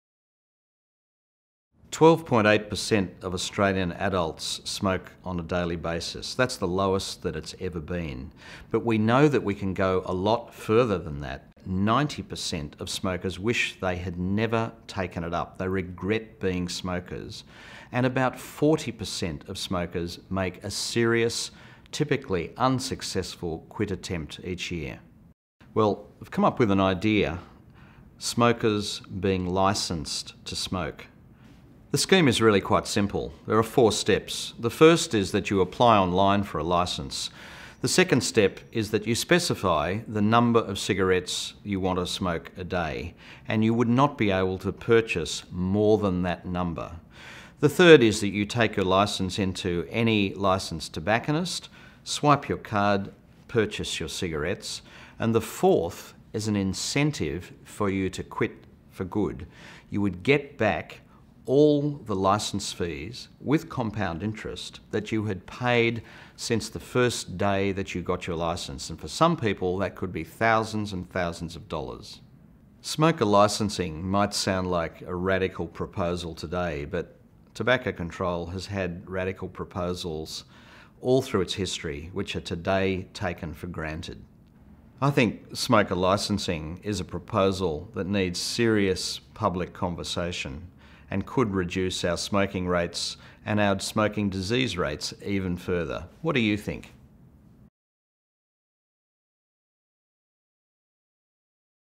Should you need a licence to smoke? Short version
Professor Simon Chapman AO from the Sydney School of Public Health discusses his radical new proposal to reduce the number of people smoking in Australia.